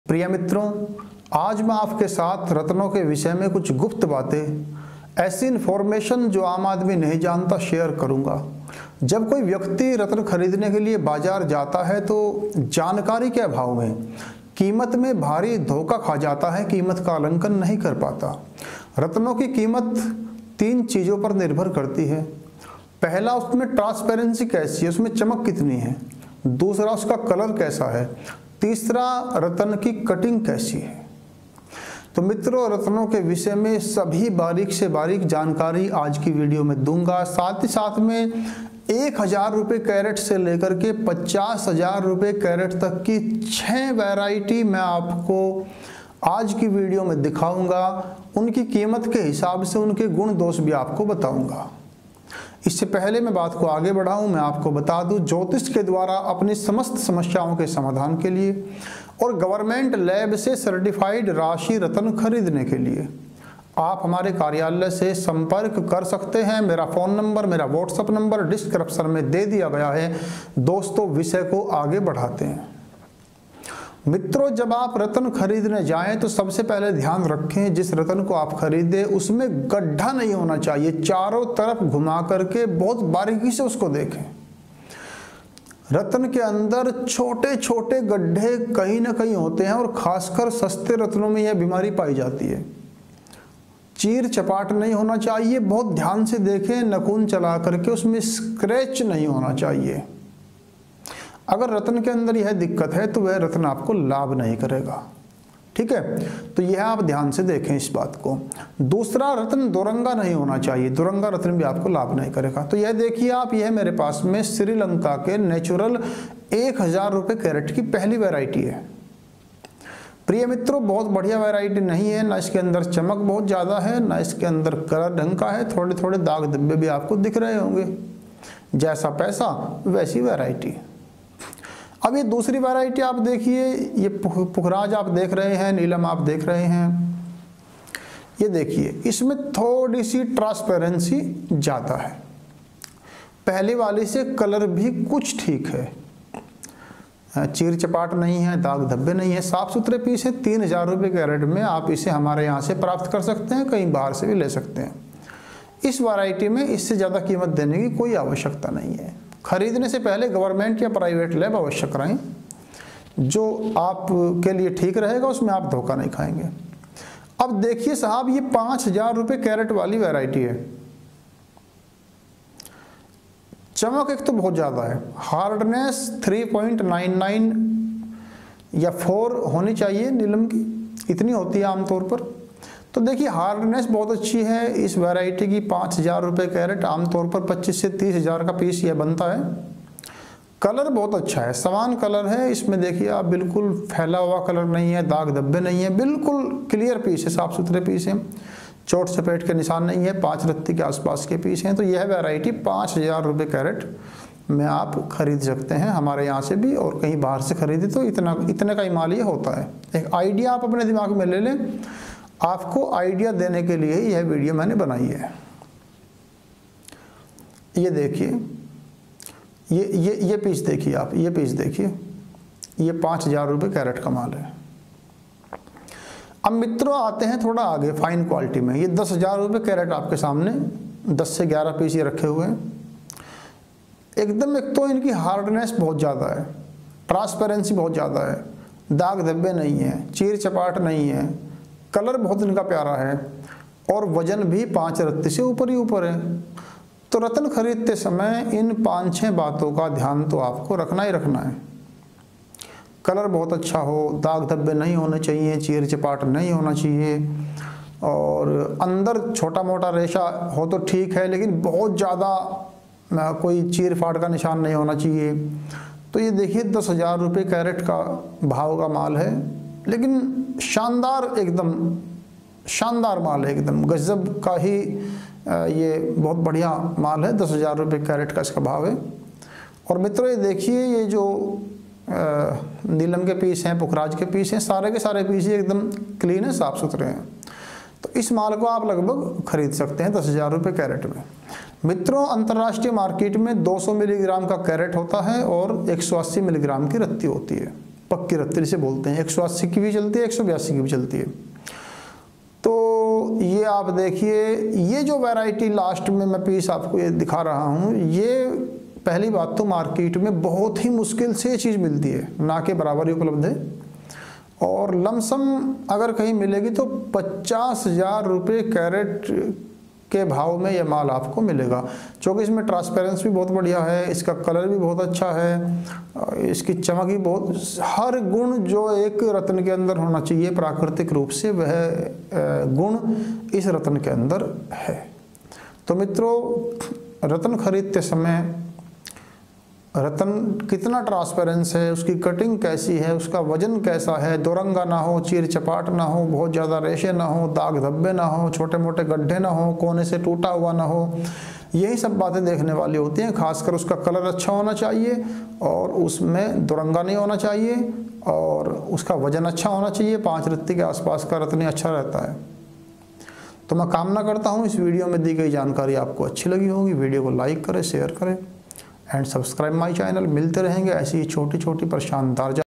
राशि रत्नों के विषय में कुछ ऐसी गुप्त बातें जो आम आदमी नहीं जानता
नमस्कार प्रिय मित्रों इस वीडियो में मैंने राशि रत्नों के विषय में कुछ ऐसी बातें बताई है जिसे आम आदमी नहीं जानता और इसी कारण वह कभी-कभी रत्नों के खरीदने में बाहरी धोखा खा जाता है मैं कामना करता हूं मेरे दर्शकों के लिए यह वीडियो काम के सिद्ध होगी my ph and whatsapp